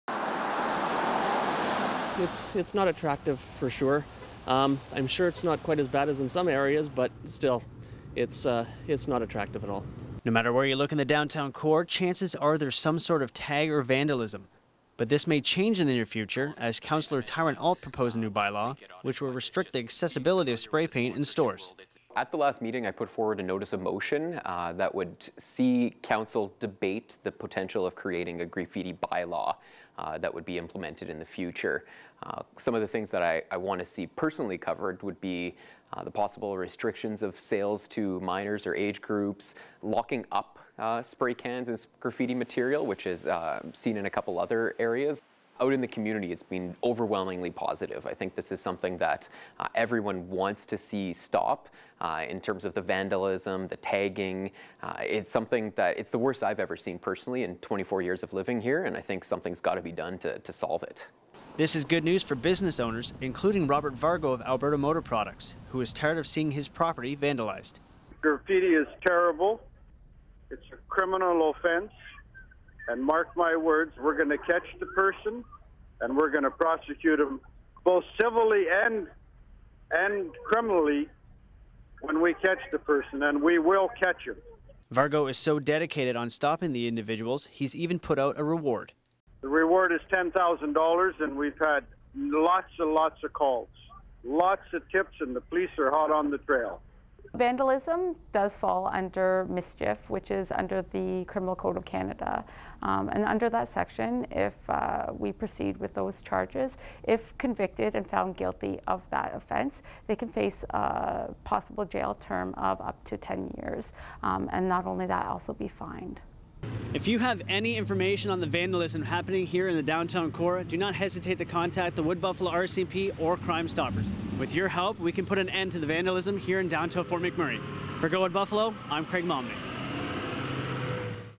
Fort McMurray Graffiti Problem
Craig Momney talks about the graffiti problem within the downtown core of Fort McMurray.